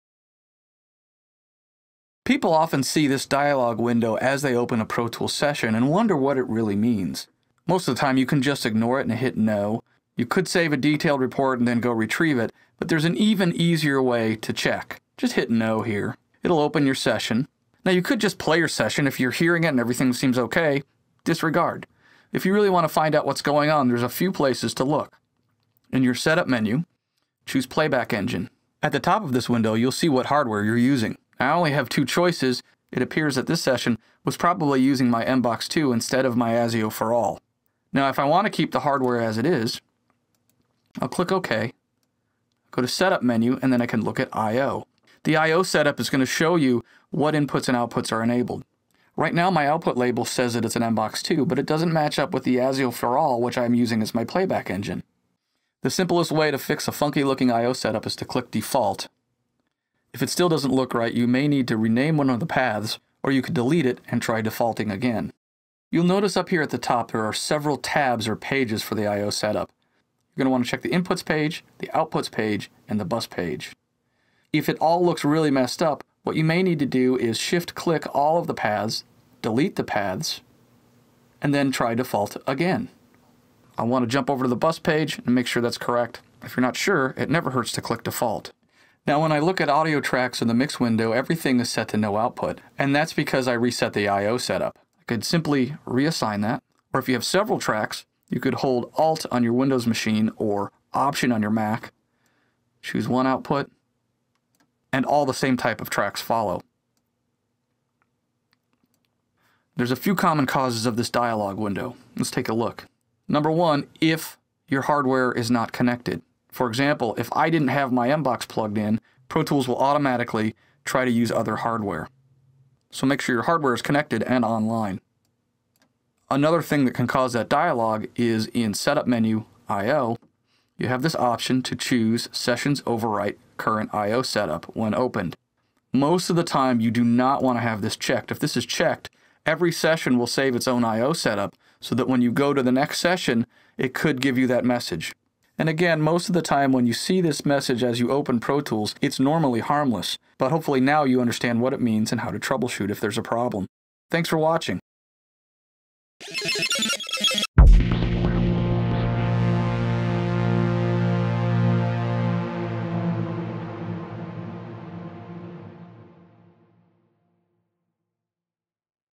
"Your IO Setup Has Changed" Alert (Quick Tips)
Learn how to deal with this message often seen when a Pro Tools session is opened.